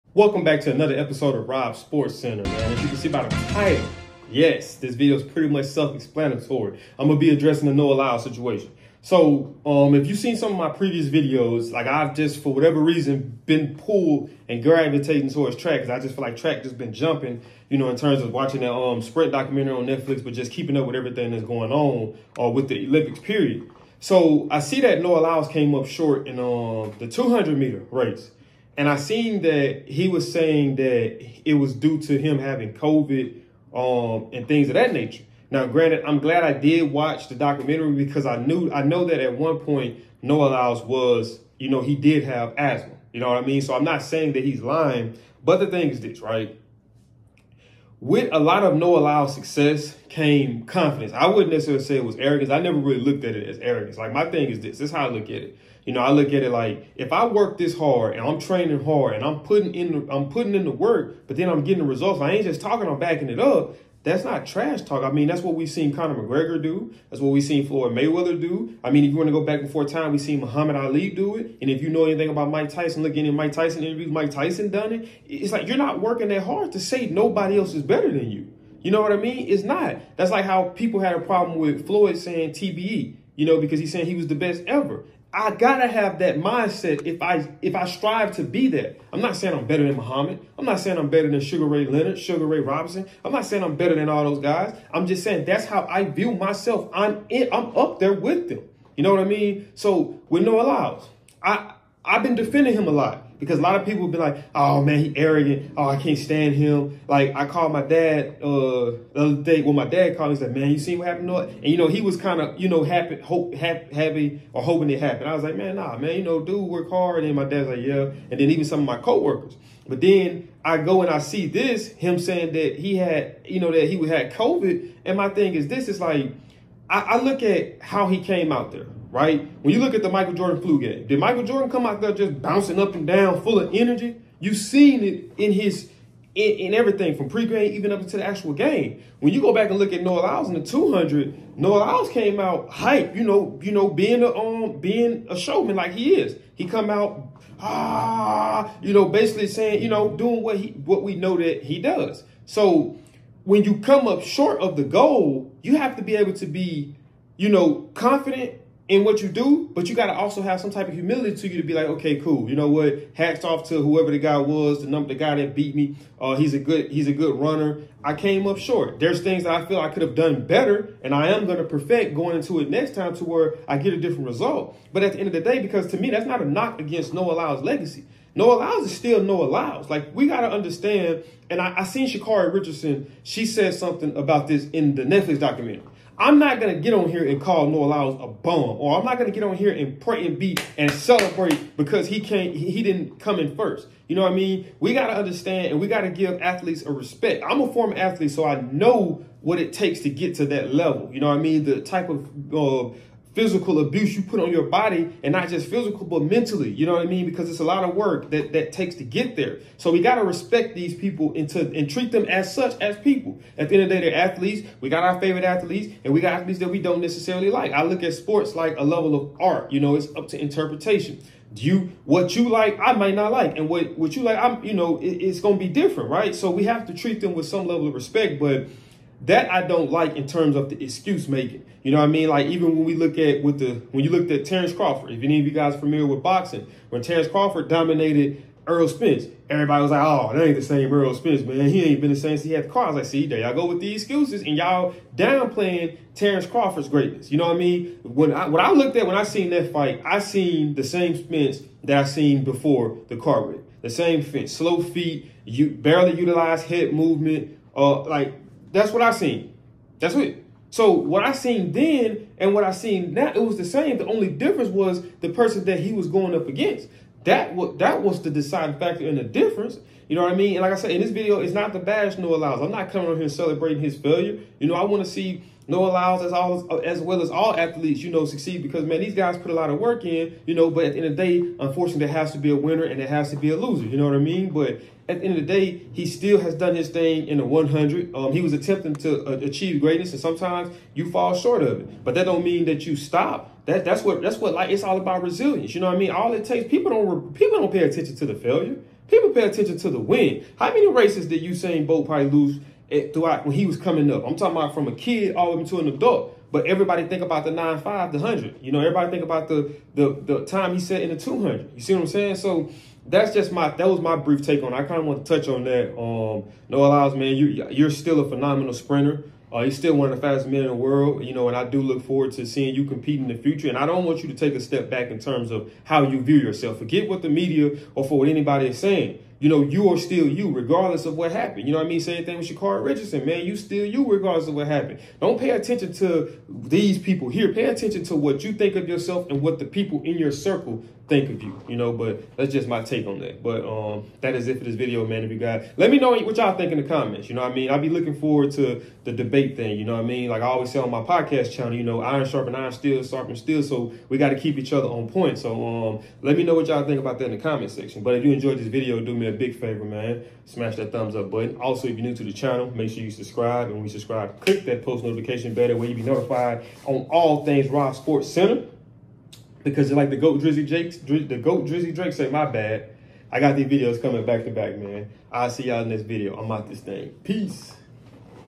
Is It COVID or Just Excuses? The Shocking Truth of Noah Lyles
In this eye-opening video, we dive deep into the world of Noah Lyles, exploring the question on everyone's mind: Is it COVID or just excuses? As one of the most prominent athletes in track and field, Noah Lyles has been making headlines with his impressive performances in the 100m and 200m events. With the Paris Olympics 2024 approaching, the pressure is on for Lyles as he represents Team USA.

Join us as we uncover the shocking truth behind Noah Lyles' recent challenges and setbacks, including his battle with COVID and its impact on his training and competition. We'll discuss his journey, his aspirations for the upcoming Olympics, and what it takes to be a gold medal contender. Plus, hear exclusive insights from his interviews and understand the man behind the medals.

Whether you're a fan of Noah Lyles, follow his journey in the NBA, or are curious about his plans for the 4x400m relay, this video is packed with essential information. Discover how Lyles is overcoming obstacles, aiming for a spot on the podium, and striving for greatness in the world of athletics. Don't miss out on this compelling look at one of the fastest men in the world. Tune in now!